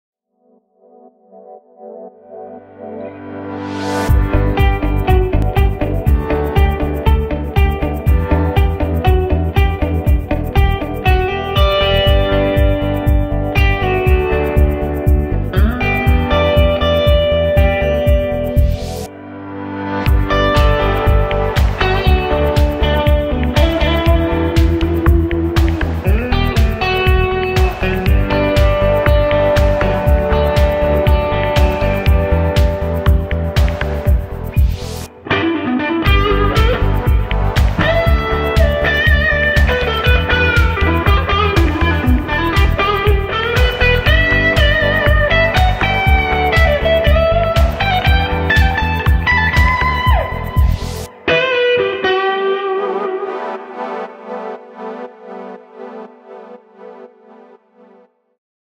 Free Backround Music NO COPYRIGHTS | Chill | Uplifting Corporate | Guitar
Video and all track played and performed by Denny Indrajaya at Arthe Camp

In this video Im using Fender stratocaster blacktop plug into Universal Audio Solo, for guitar sound im using UAD Diesel VH4, reverbs and delay using Logic Pro X stocks, for instruments all using Logic Pro X VSTi and some samples on my library, music arrangements and all tracks was originally played and performed by me, mixed and mastered all in the box using some UAD plugins, Waves and Logic Pro X stocks plugins.

Please enjoy, the song, sounds, and also the view, God bless you.

Original track